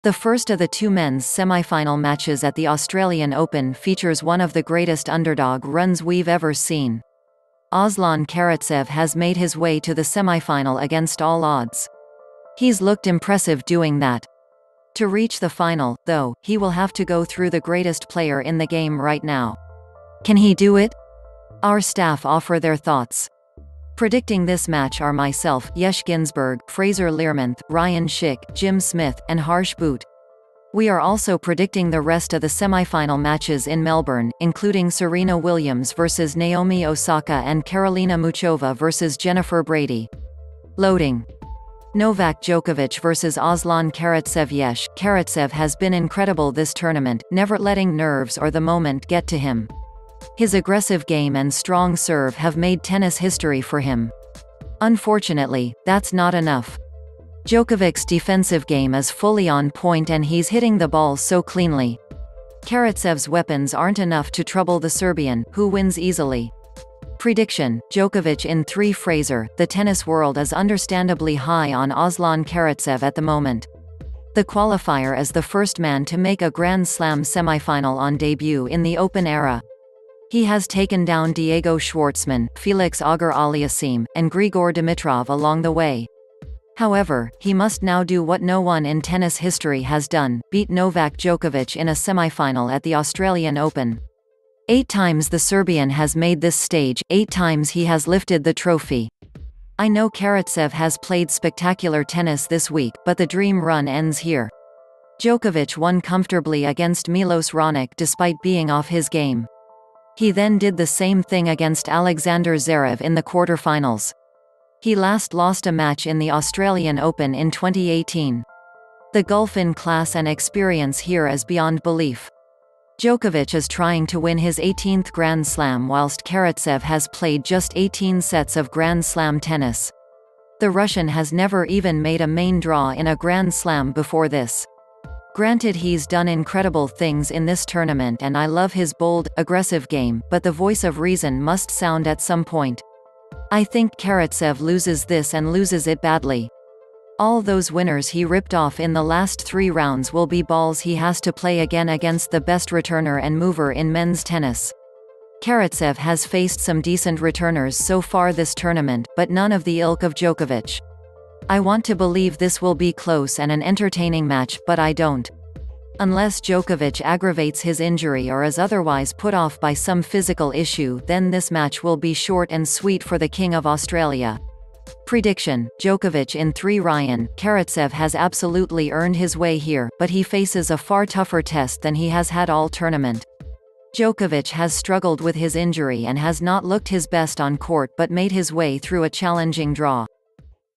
Australian Open Semifinal Predictions Djokovic vs Karatsev
The first of the two men’s semifinal matches at the Australian Open features one of the greatest underdog runs we’ve ever seen. Aslan Karatsev has made his way to the semifinal against all odds. He’s looked impressive doing that. To reach the final, though, he will have to go through the greatest player in the game right now. Can he do it? Our staff offer their thoughts. Predicting this match are myself (Yesh Ginsburg), Fraser Learmonth, Ryan Schick, Jim Smith, and Harsh Bhoot. We are also predicting the rest of the semifinal matches in Melbourne, including Serena Williams vs Naomi Osaka and Karolina Muchova vs Jennifer Brady. Loading... Novak Djokovic vs Aslan KaratsevYesh:Karatsev has been incredible this tournament, never letting nerves or the moment get to him. His aggressive game and strong serve have made tennis history for him. Unfortunately, that’s not enough. Djokovic’s defensive game is fully on point and he’s hitting the ball so cleanly. Karatsev’s weapons aren’t enough to trouble the Serbian, who wins easily. Prediction: Djokovic in 3Fraser:The tennis world is understandably high on Aslan Karatsev at the moment. The qualifier is the first man to make a Grand Slam semifinal on debut in the Open Era. He has taken down Diego Schwartzman, Felix Auger-Aliassime, and Grigor Dimitrov along the way. However, he must now do what no one in tennis history has done: beat Novak Djokovic in a semifinal at the Australian Open. Eight times the Serbian has made this stage, eight times he has lifted the trophy. I know Karatsev has played spectacular tennis this week, but the dream run ends here. Djokovic won comfortably against Milos Raonic despite being off his game. He then did the same thing against Alexander Zverev in the quarterfinals. He last lost a match in the Australian Open in 2018. The gulf in class and experience here is beyond belief. Djokovic is trying to win his 18th Grand Slam whilst Karatsev has played just 18 sets of Grand Slam tennis. The Russian has never even made a main draw in a Grand Slam before this. Granted he’s done incredible things in this tournament and I love his bold, aggressive game, but the voice of reason must sound at some point. I think Karatsev loses this and loses it badly. All those winners he ripped off in the last three rounds will be balls he has to play again against the best returner and mover in men’s tennis. Karatsev has faced some decent returners so far this tournament, but none of the ilk of Djokovic. I want to believe this will be close and an entertaining match, but I don’t. Unless Djokovic aggrevates his injury or is otherwise put off by some physical issue then this match will be short and sweet for the King of Australia. Prediction: Djokovic in 3Ryan:Karatsev has absolutely earned his way here, but he faces a far tougher test than he has had all tournament. Djokovic has struggled with his injury and has not looked his best on court but made his way through a challenging draw.